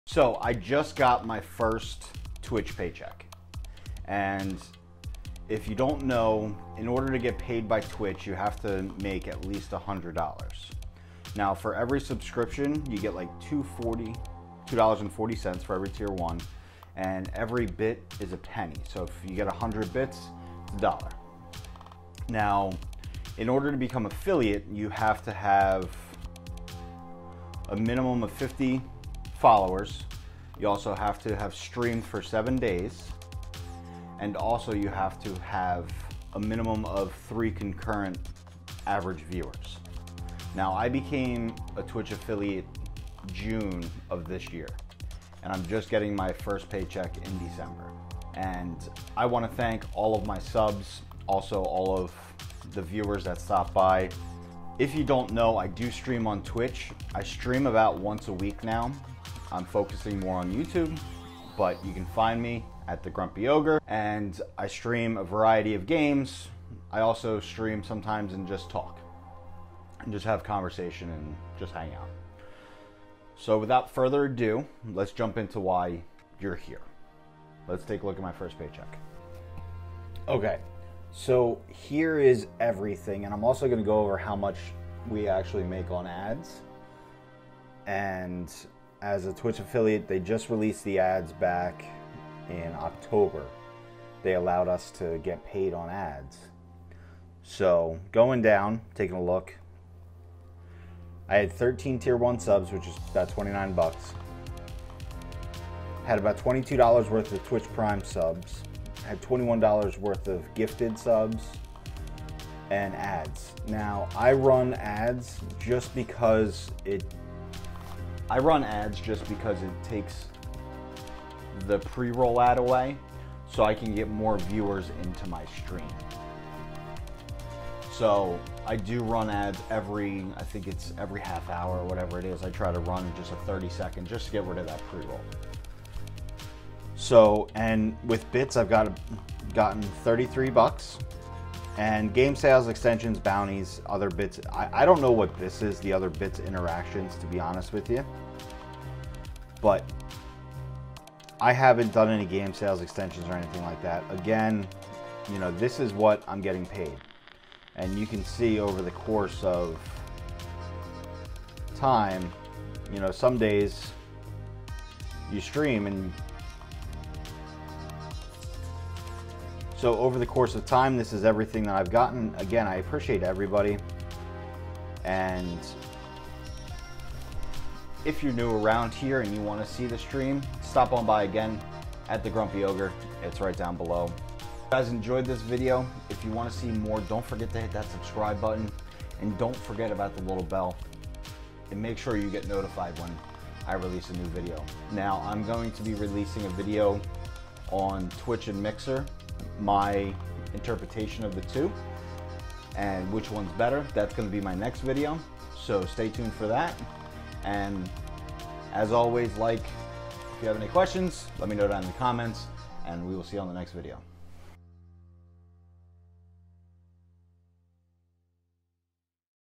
MY TWITCH INCOME|How much does a small streamer get paid?
MY FIRST TWITCH PAYCHECK
How much does a small streamer get paid?

Twitch.tv/the_grumpy_ogre